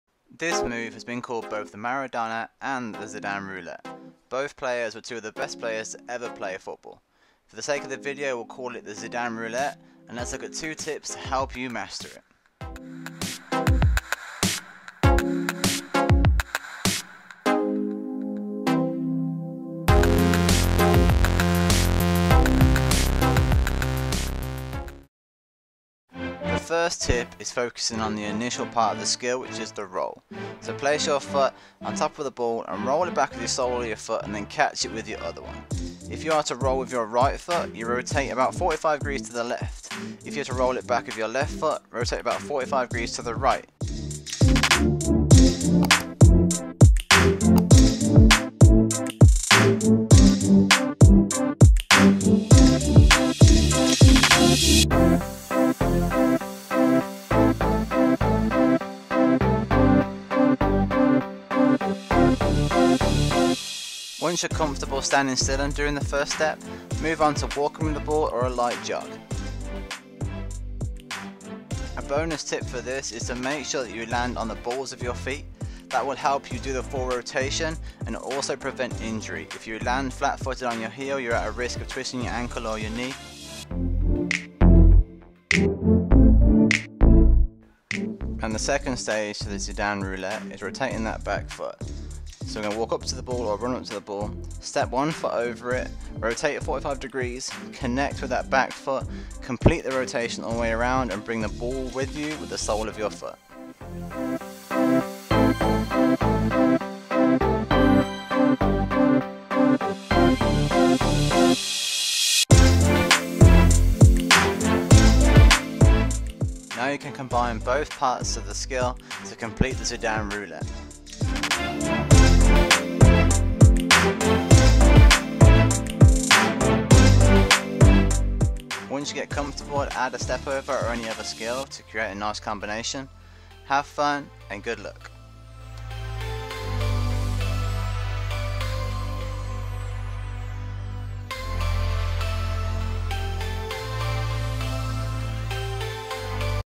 Learn The Zidane Roulette | 2020 Tutorial
The Zidane roulette is also called the Maradona. Both Zidane & Maradona were world class footballers playing in different times, but both dominating the pitch.
In this video we call the skill the Zidane roulette, and we show two tips to help you master the skill. Learning how to do the Zidane roulette is as simple as two steps.
Always practice with both feet - if you can perform the Zidane roulette with both feet, it will give you a huge advantage in keeping control when in a congested situation.

Work hard, have fun & good luck!